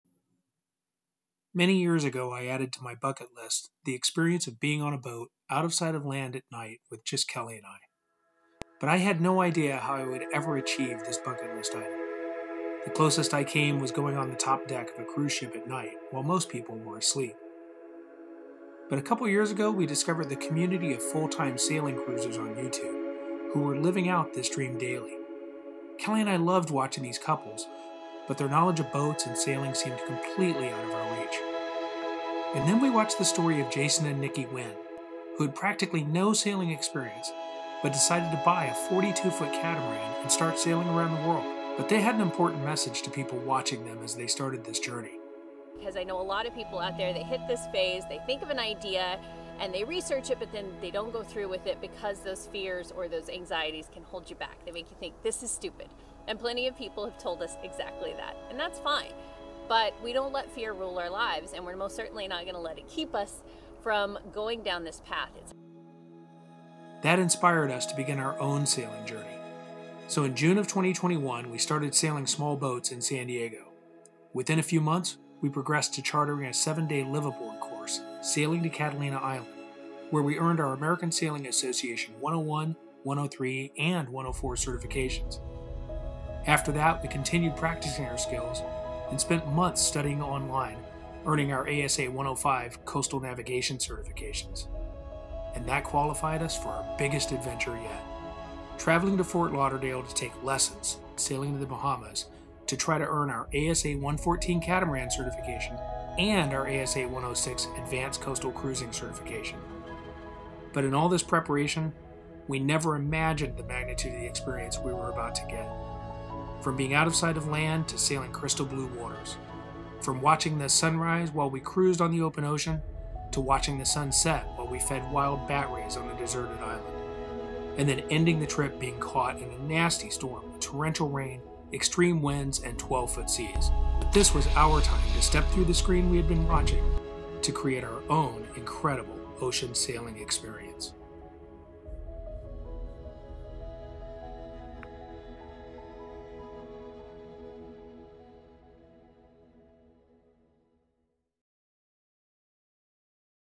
Sailing to the Bahamas (Preview Trailer)
This is the preview of our upcoming video telling the story of how we went from almost no sailing experience to sailing roundtrip from Fort Lauderdale to the Bahamas. In less than 18 months, we earned several American Sailing Association (ASA) certifications, culminating in this incredible trip where we experienced the beauty of sailing in the Bahamas, and the drama of getting caught in a storm while crossing the Gulf Stream. Full edit will be coming soon!